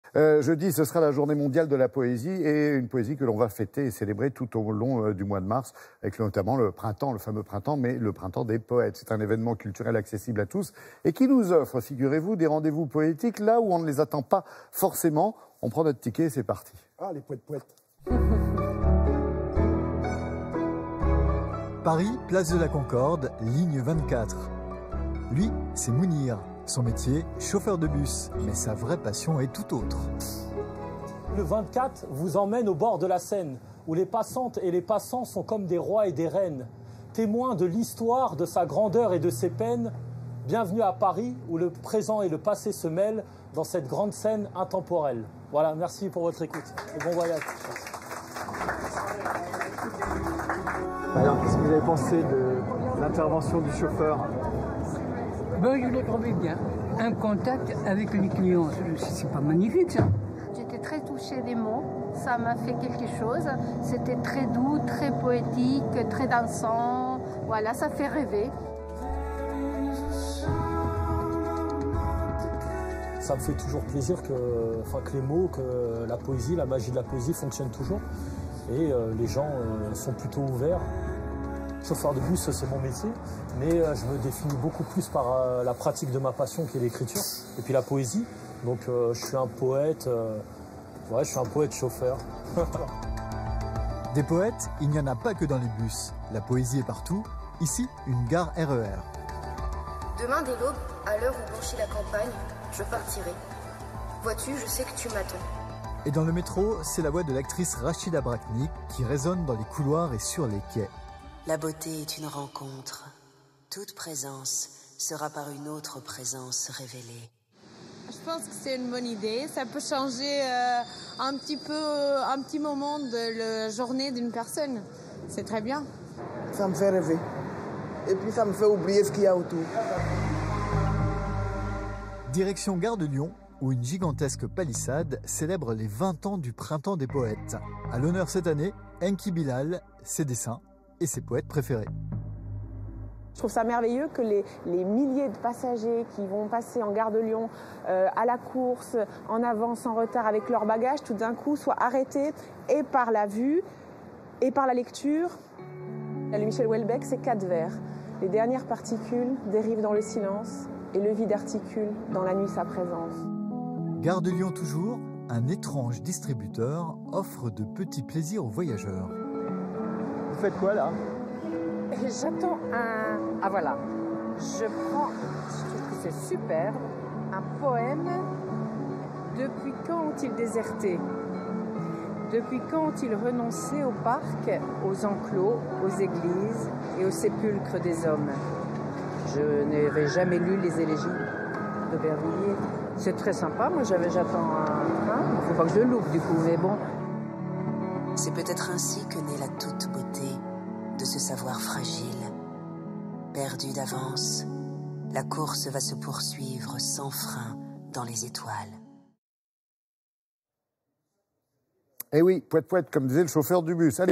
Actu - La RATP tout un poème
Chroniqueur : Laurent Bignolas

Pour la cinquième année consécutive, la RATP organise une nouvelle édition du Grand Prix Poésie, destiné à révéler des poètes amateurs de tous âges.
Enfants, jeunes et adultes sont invités à participer à ce grand concours d’écriture poétique, du 13 mars au 14 avril 2019.
Pour cette nouvelle édition, le jury du Grand Prix sera présidé par la comédienne et romancière Isabelle Carré.